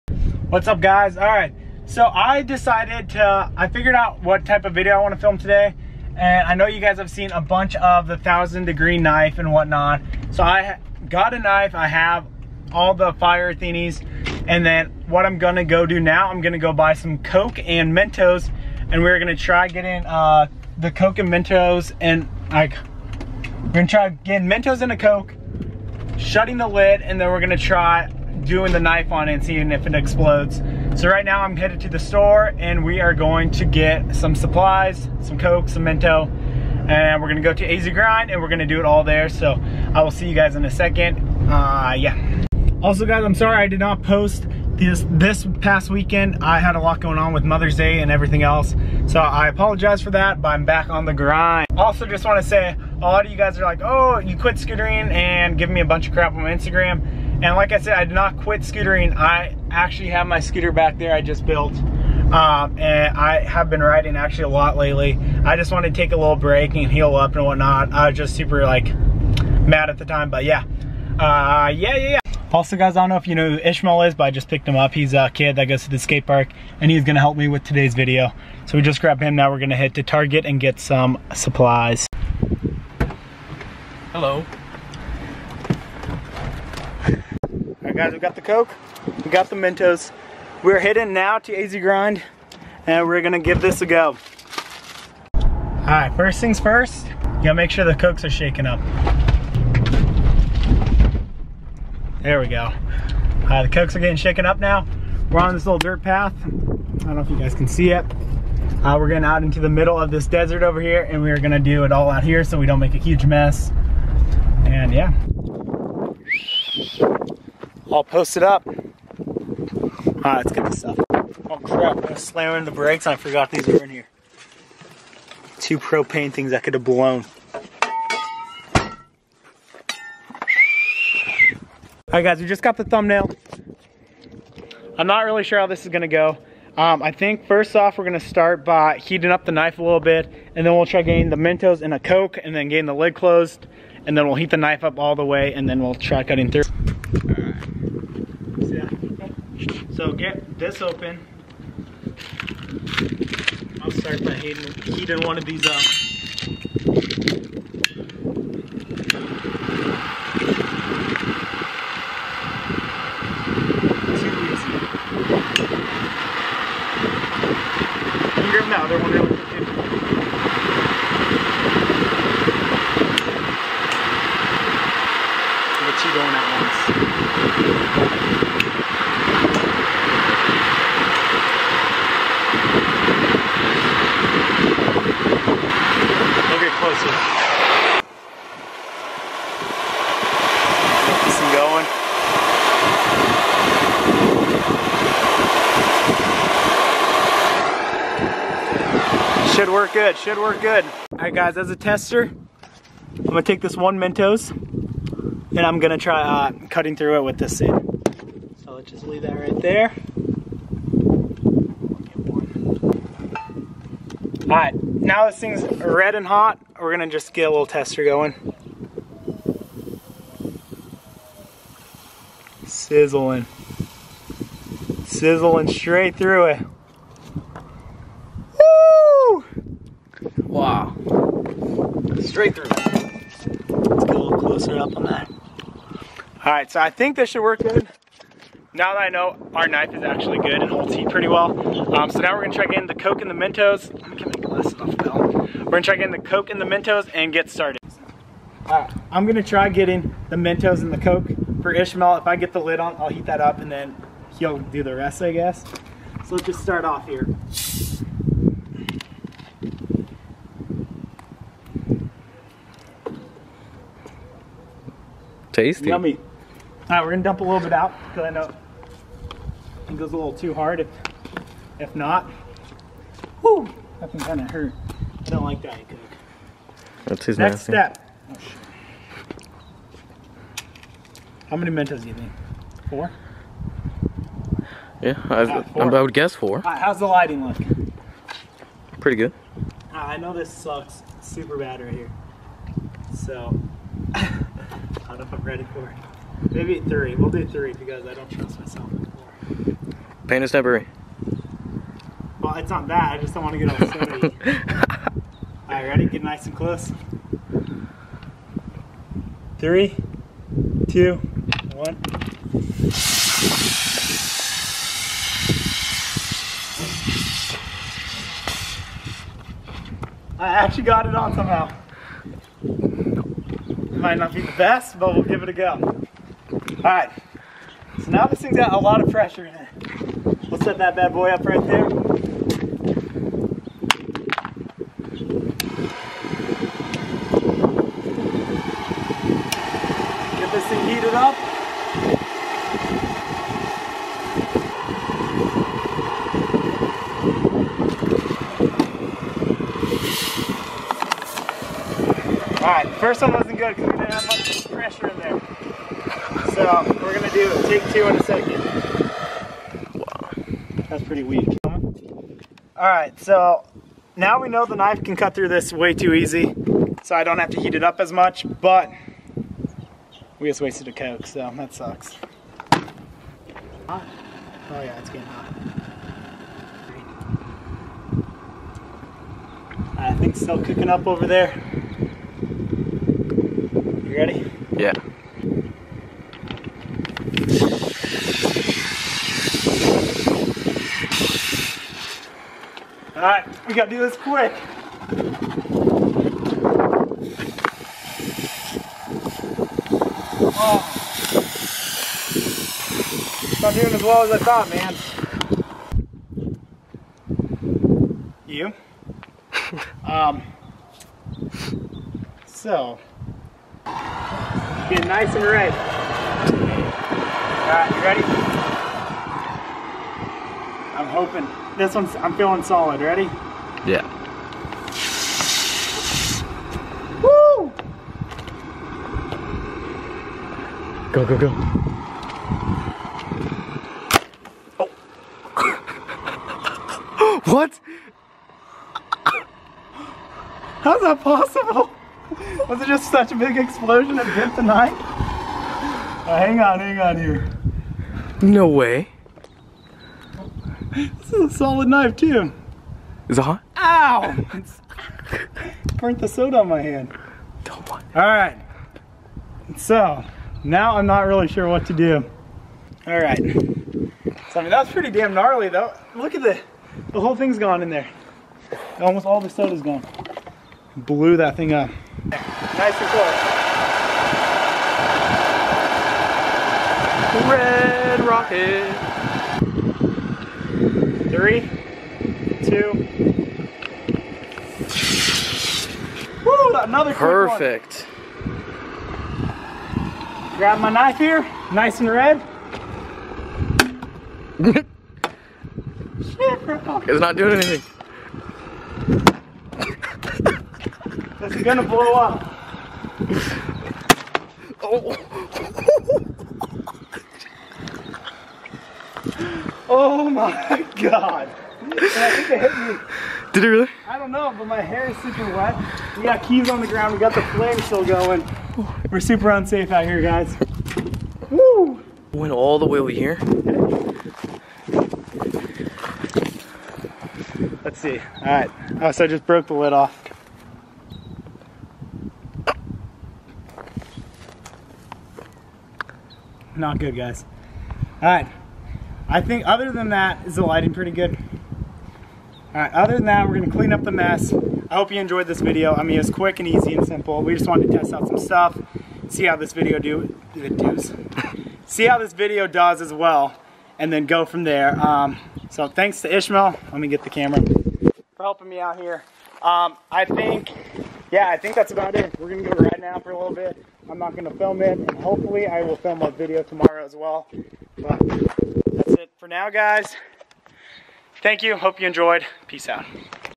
1000 DEGREE KNIFE VS COKE & MENTOS.... EXPLOSION
EXPERIMENT, this idea came to me and I always wanted to try it to see what would happen so today was that day!!! Remember this was a experiment so it didnt go exactly as planned but we still made it work! Hope you enjoyed this video and hopefully will get some more out there soon for you all!!! Thanks for your guys' support I truly do love it!